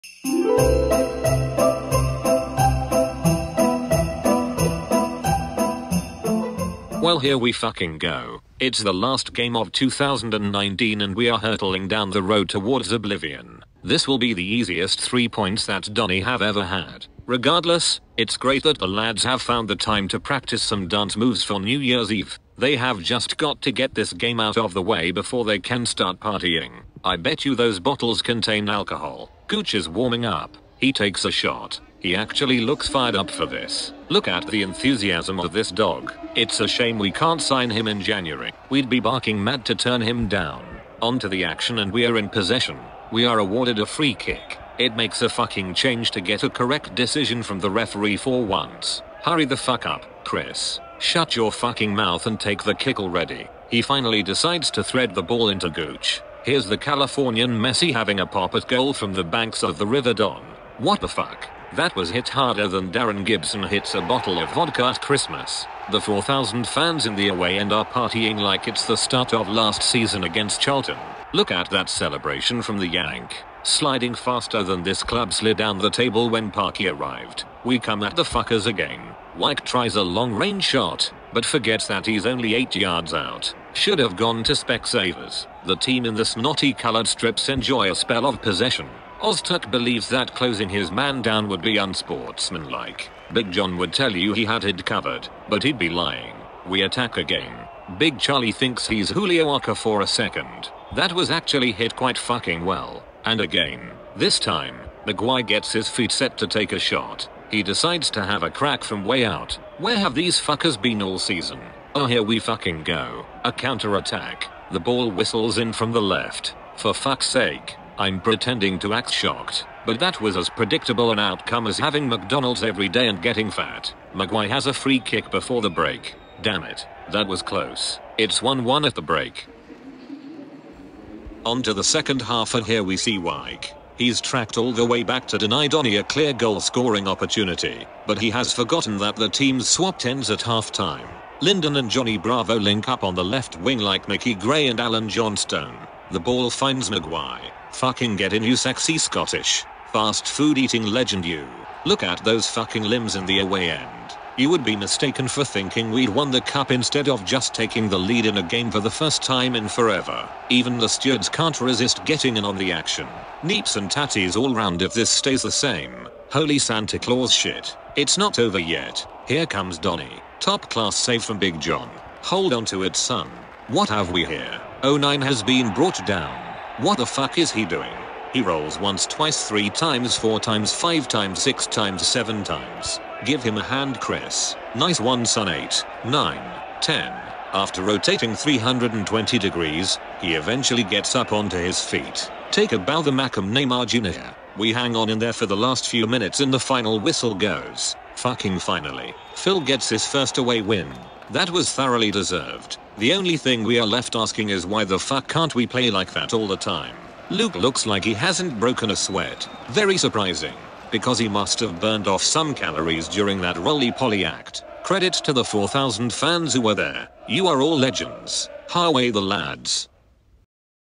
Doncaster Rovers v Sunderland AFC (Alternative Commentary)
Alternative Commentary from SAFC's recent visit to Doncaster.